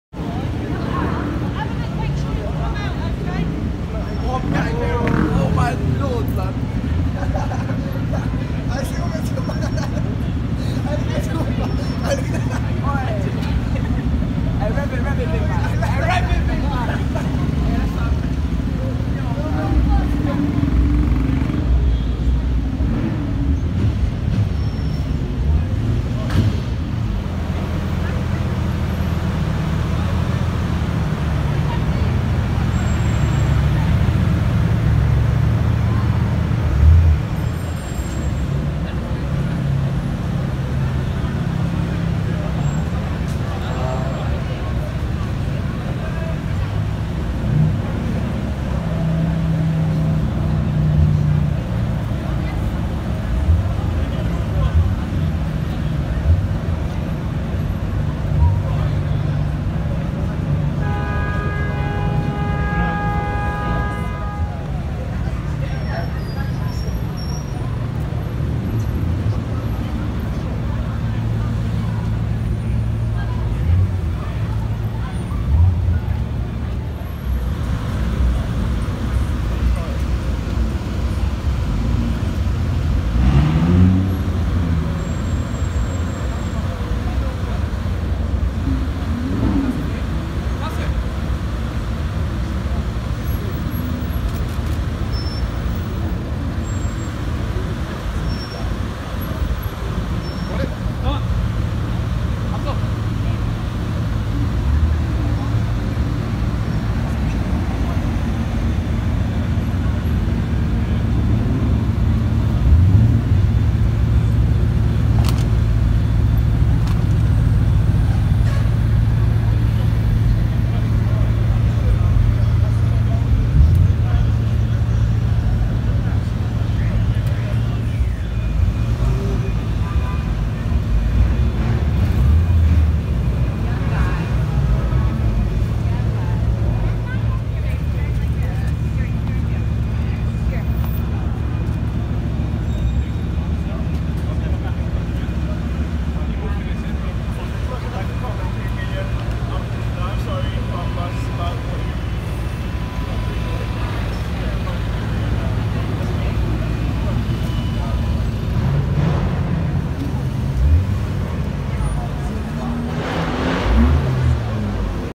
Famous ‘F1’ Veyron attracts crowds in London
The famous ~£10,000,000 F1 plate.